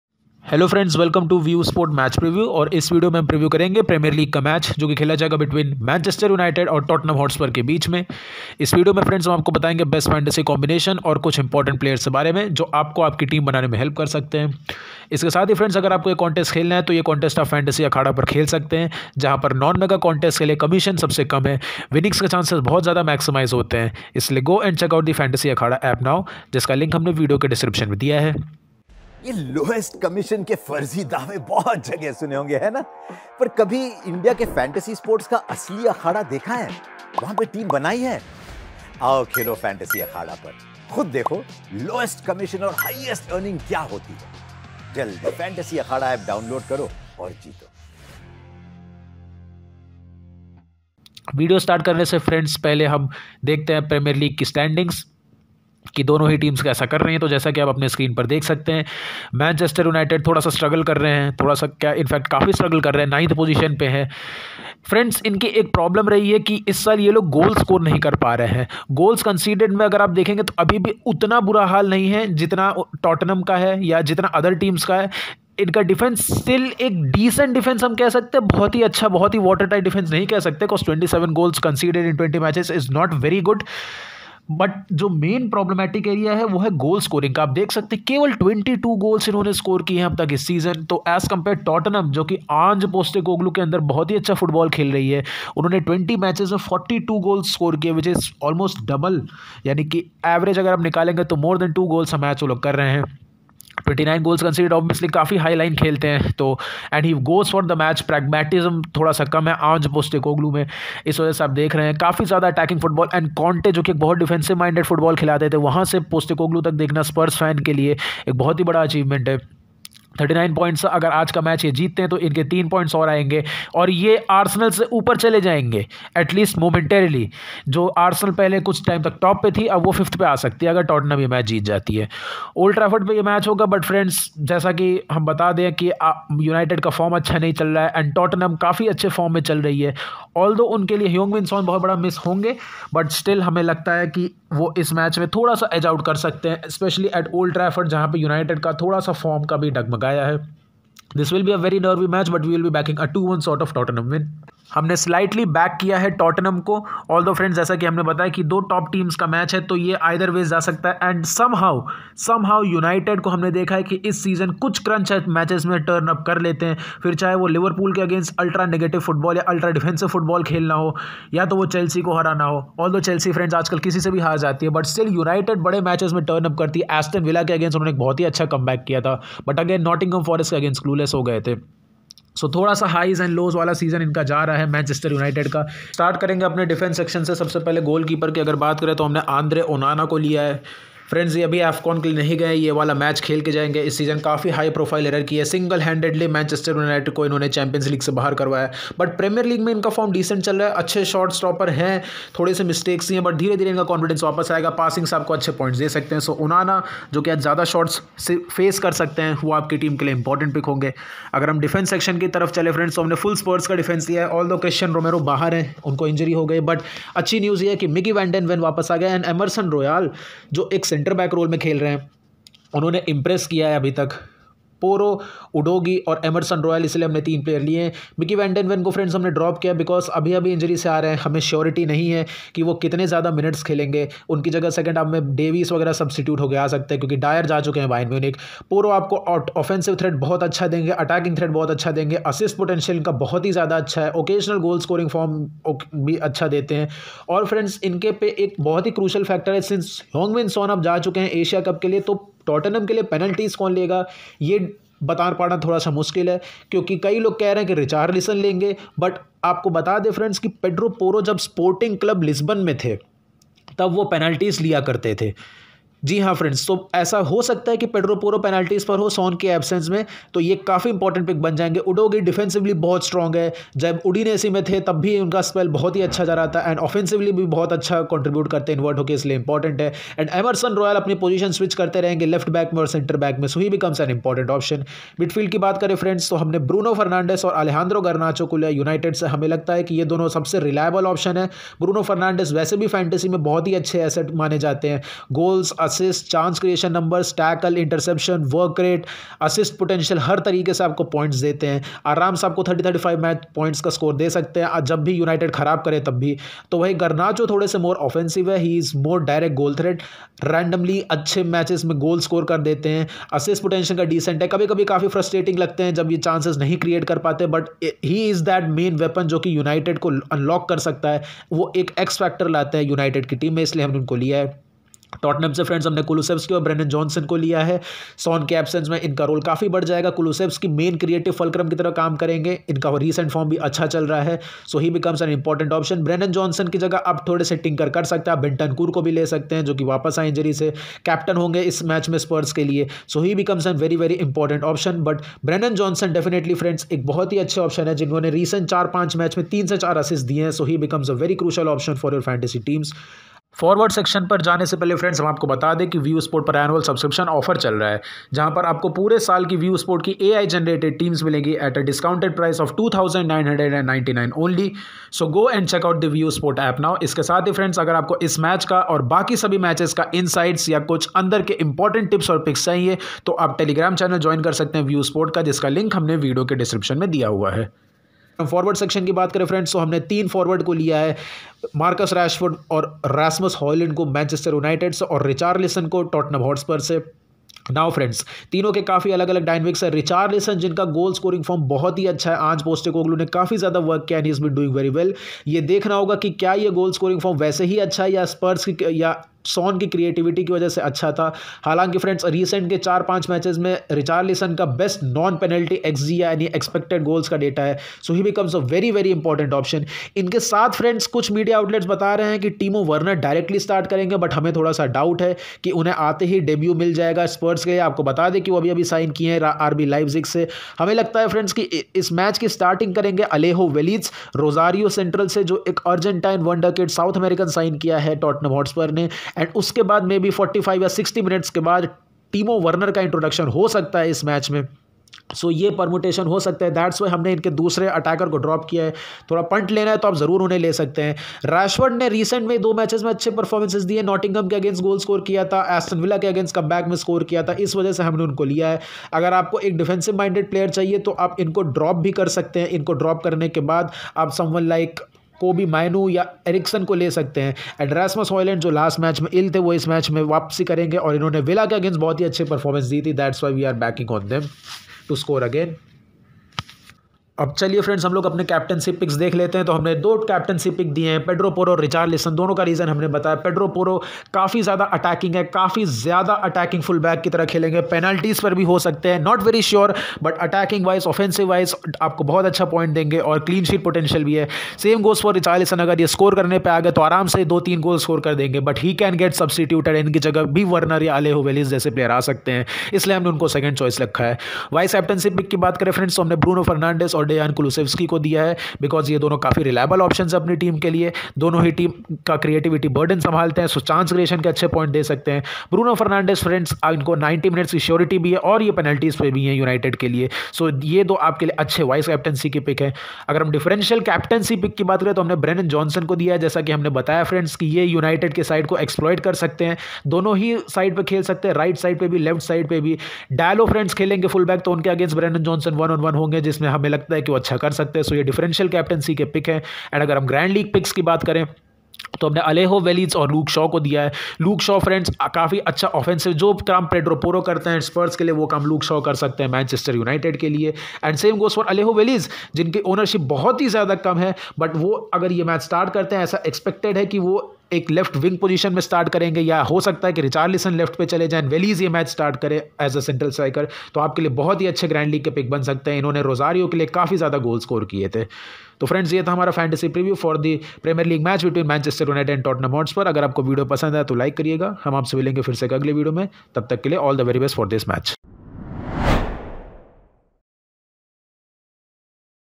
MUN vs TOT Dream11 Prediction | Manchester United vs Tottenham Hotspur | Premier League Preview
Get ready for the most awaited Premier League clash between Manchester United vs Tottenham Hotspur.

In this video, we provide a detailed match preview of the MUN vs TOT game, including our predictions, analysis, and insights. We also discuss the probable lineups, player performance, and tactical analysis to give you a complete picture of the upcoming fixture.